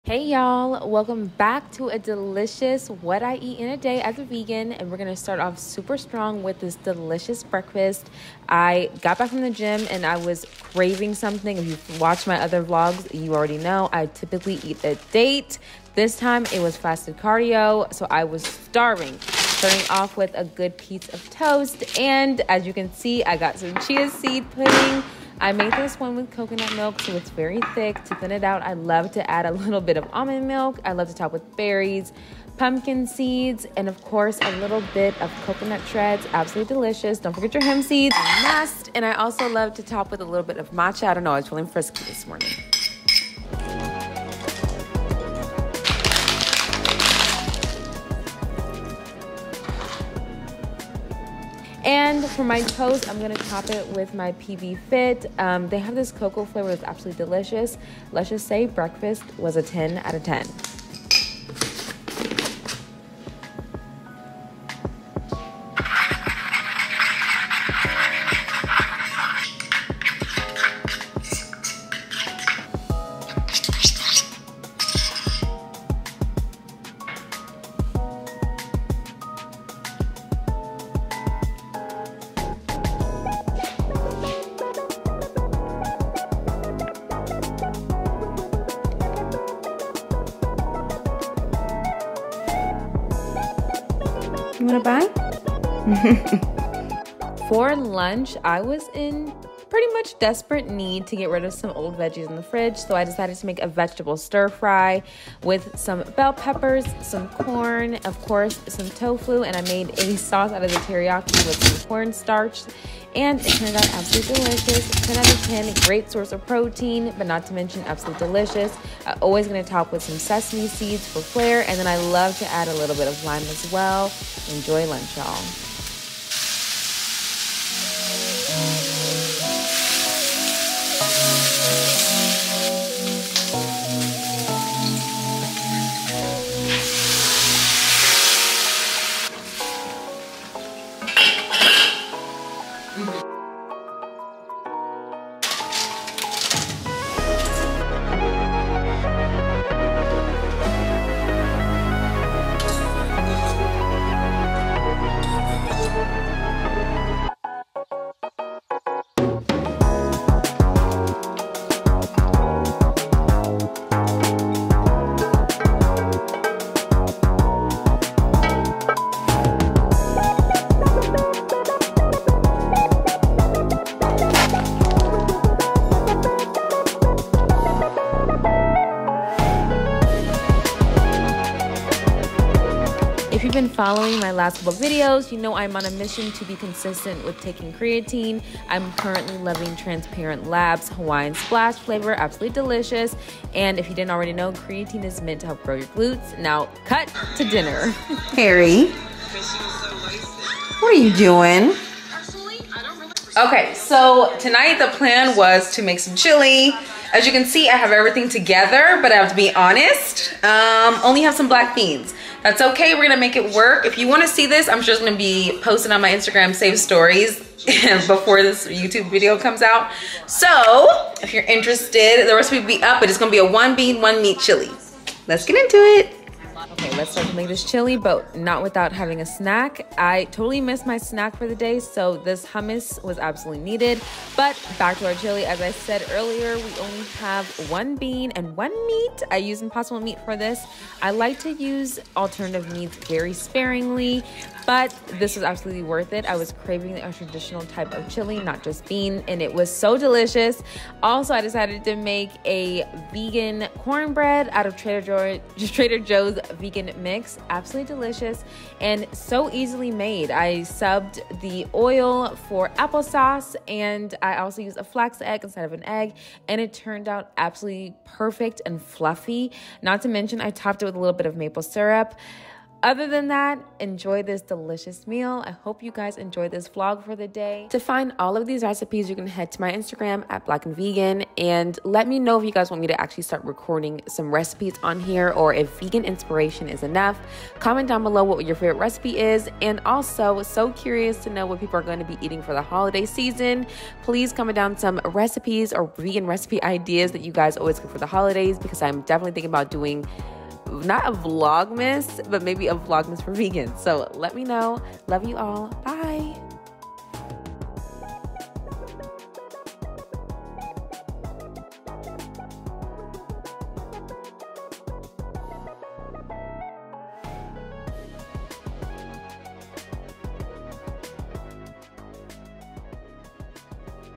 WHAT I EAT IN A DAY AS A VEGAN (Meal Inspiration + Protein Packed Meals)
Hey y'all! Back with a What I Eat in a Day as an active vegan. Let me know what dish is your fav or what you wanna see.

What I Eat INNNNA has hit Youtube. Here's how I tackle 90g of plant based protein on a cut! Enjoy!